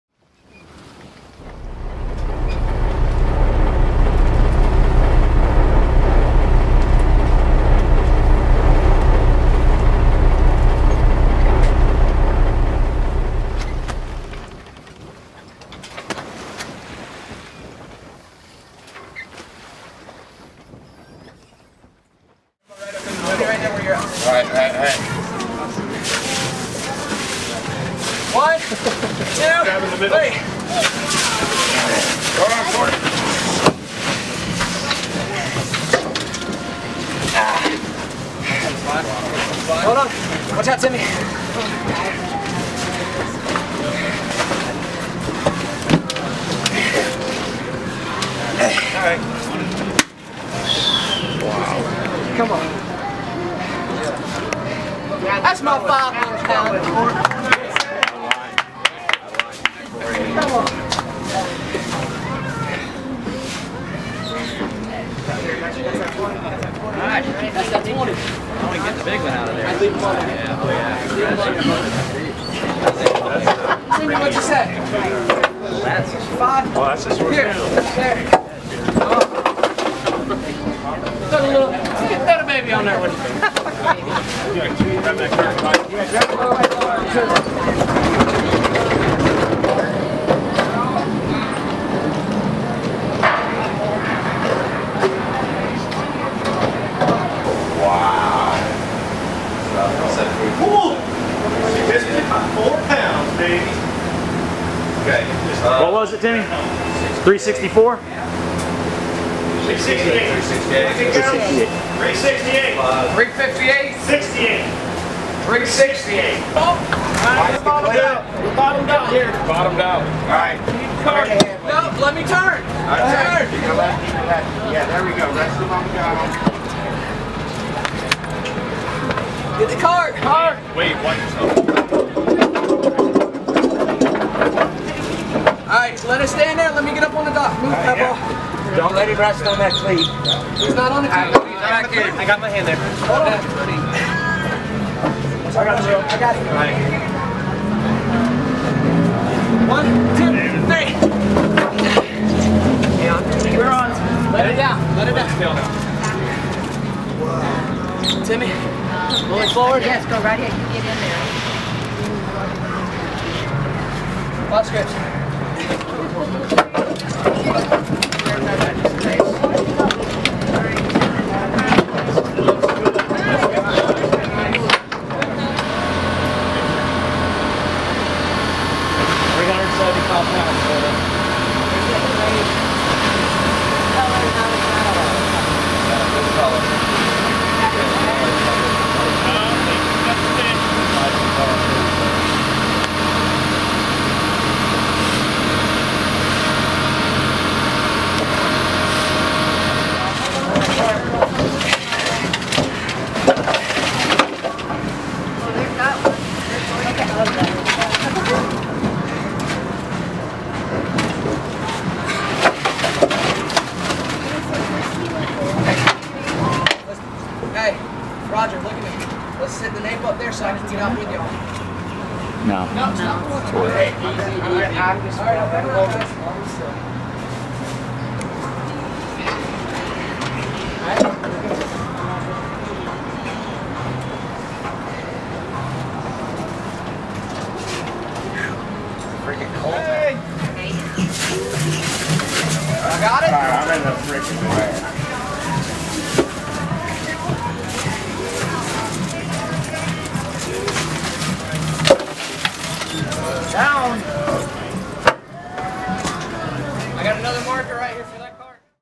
Massive Bluefin Tuna and Swordfish, Port Orange Florida
Massive Bluefin Tuna and Swordfish, Port Orange Florida. a 14 day fishing trip over 400 miles out to sea landed this crew a massive catch.